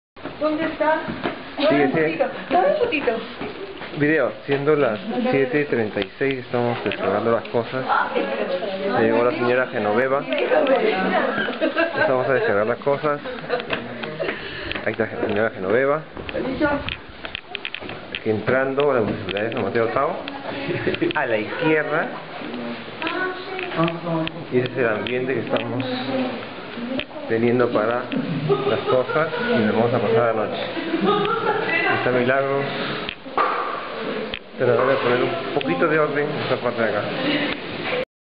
En la municipalidad de San Mateo de Otao
Esperamos hasta que llegó la sra. Genoveva para abrirnos las puertas de la municipalidad para dejar las donaciones y ver los lugares donde pasaríamos la noche.